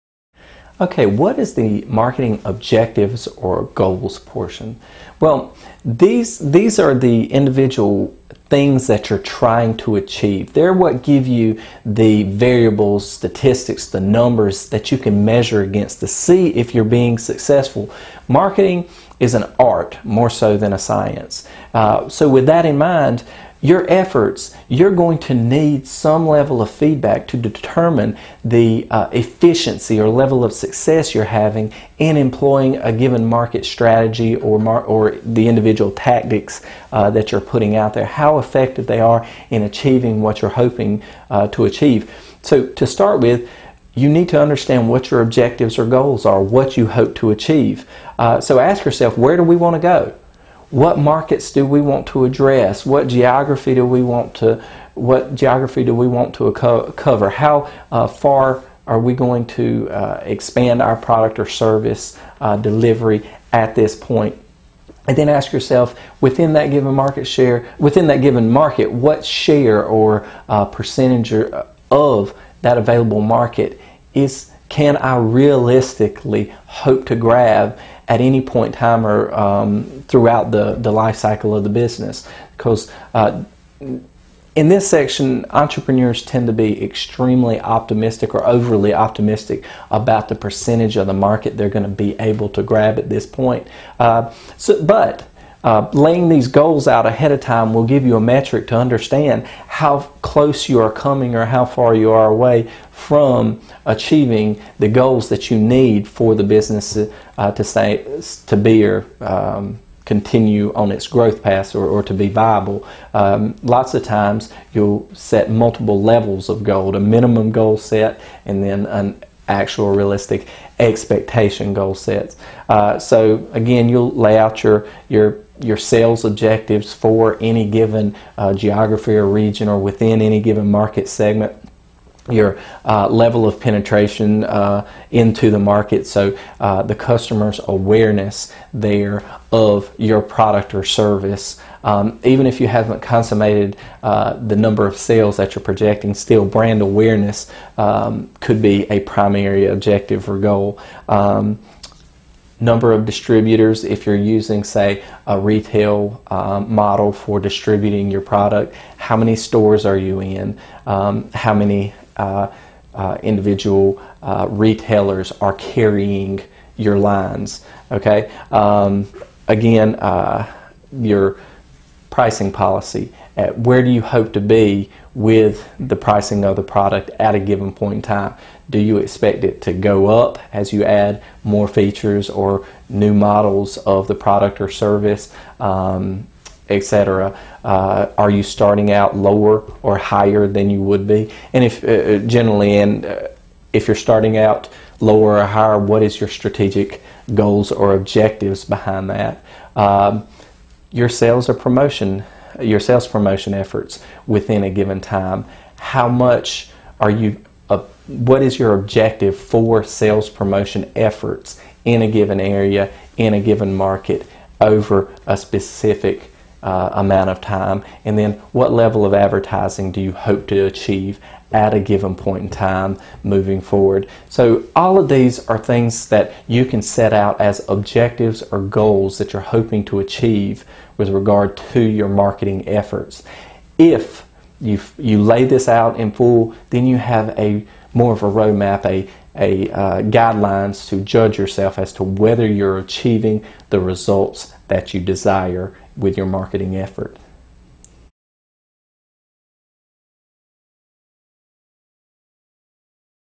Marketing Plan Goals and Objectives Section
This video provides a brief introduction to the goals and objectives section of the marketing plan.